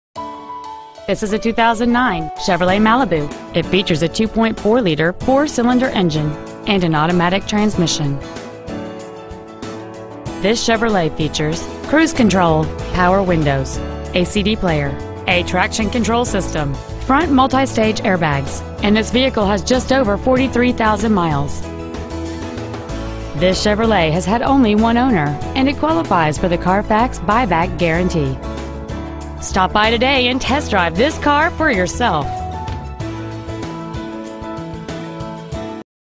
2009 Chevrolet Malibu Stockton
Year: 2009
 Make: Chevrolet
 Model: Malibu
 Engine: 4-cyl. 2.4 liter
 Trans.: automatic
 Exterior: silver
 Miles: 43,206
 Interior: gray

 9650 Auto Center Dr.

 2009 Chevrolet Malibu Elk Grove CA